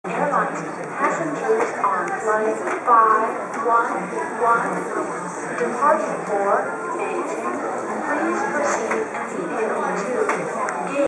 台湾 台北桃園国際空港 第2ターミナル 大量のソニーブラビア
台湾 台北桃園国際空港 第2ターミナルに大量にあったソニーの液晶テレビブラビアです。